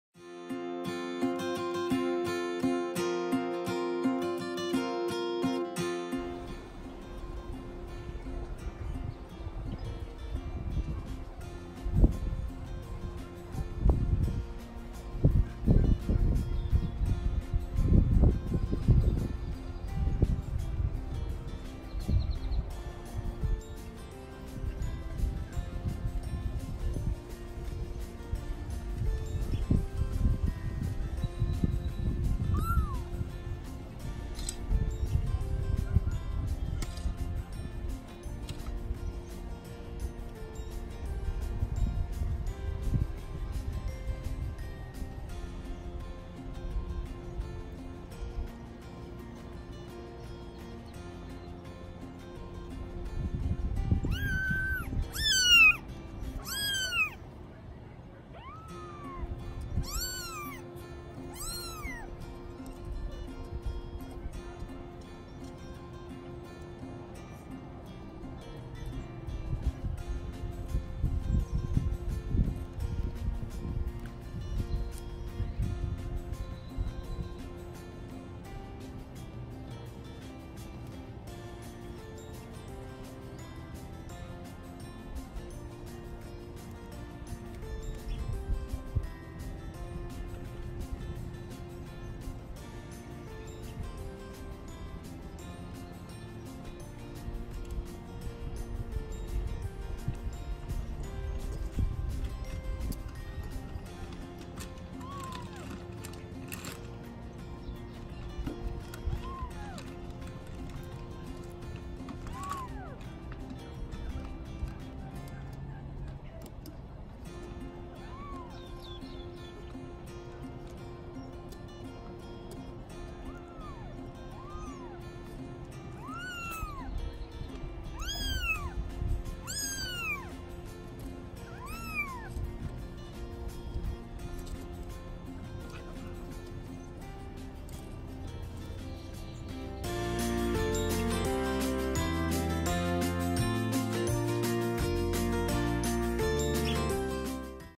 Cute Meowing Baby Kitties 8 ft. Off the Ground!
Updating the 3 kitties that are still 8 feet off the ground!  They are looking good and healthy!  They're about 3 weeks here.  Mama Orange has been feeding them well!